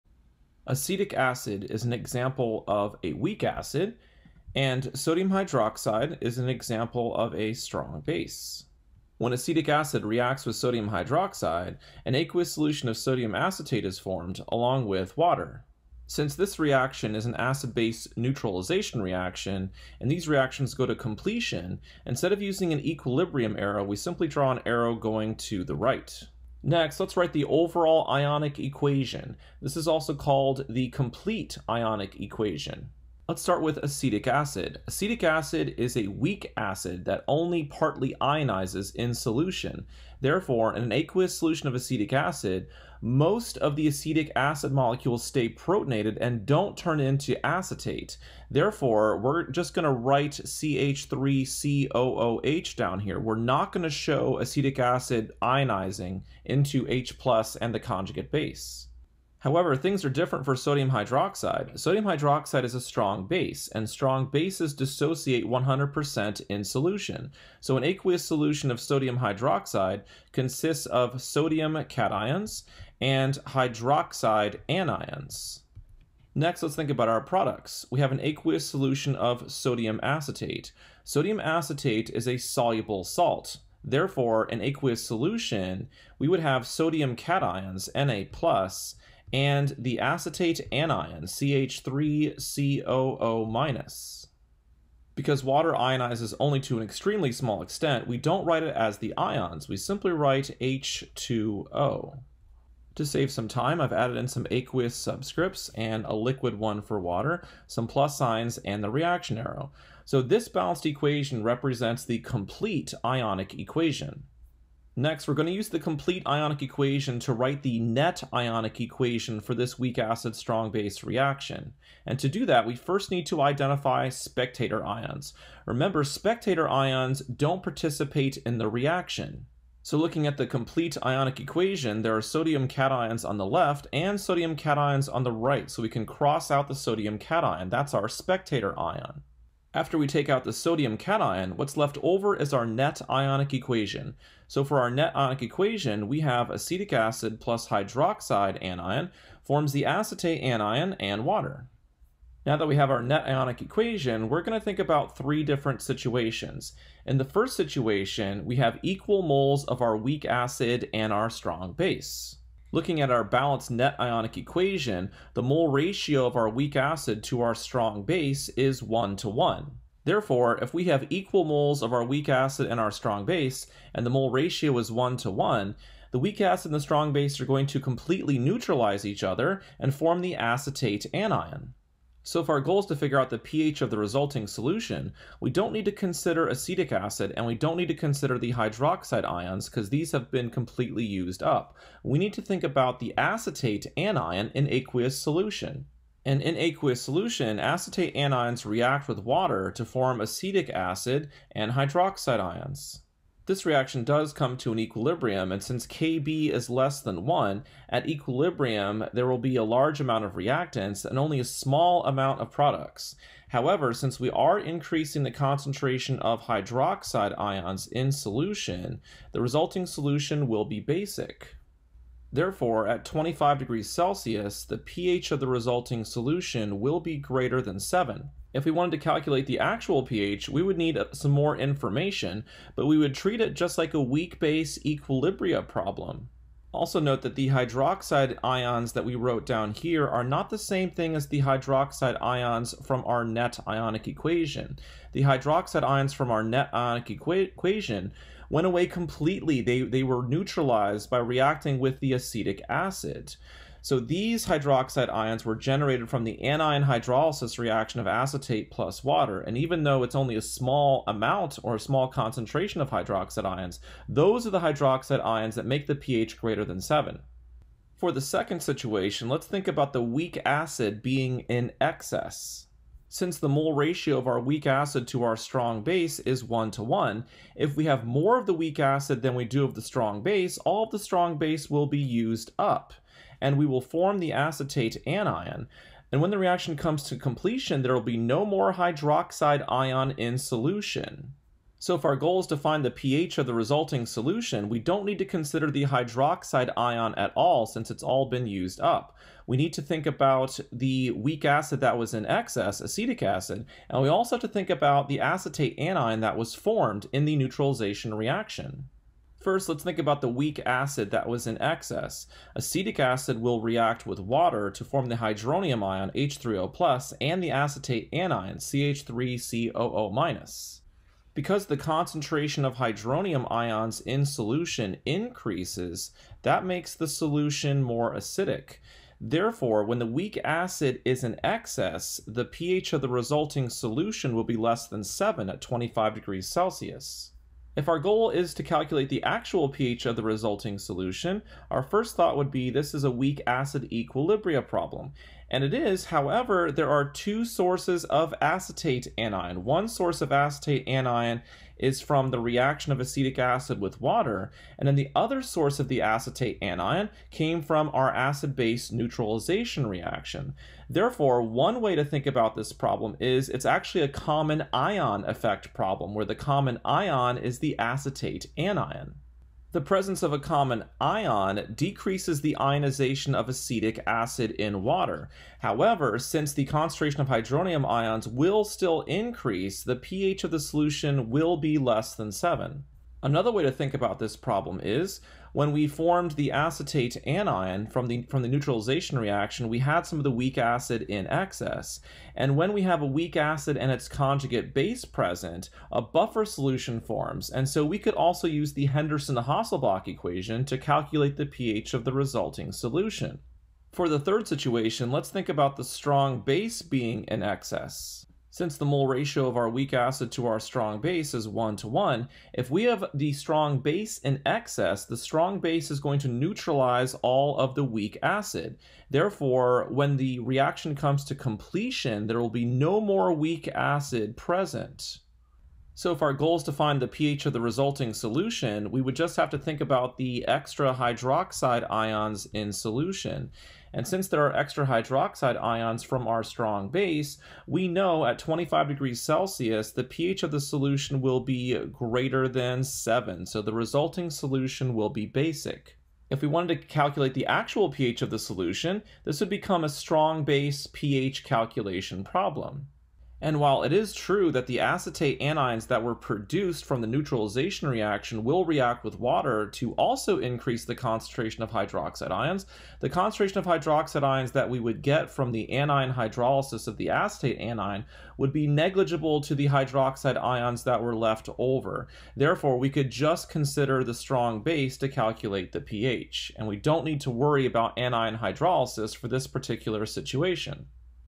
Weak acid–strong base reactions | Acids and bases | AP Chemistry | Khan Academy
When a weak acid and a strong base are mixed, they react according to the following net-ionic equation: HA(aq) + OH⁻(aq) → A⁻(aq) + H₂O(l). If the acid and base are equimolar, the pH of the resulting solution can be determined by considering the equilibrium reaction of A⁻ with water. If the acid is in excess, the pH can be determined from the concentrations of HA and A⁻ after the reaction. If the base is in excess, the pH can be determined from the concentration of excess OH⁻. View more lessons or practice this subject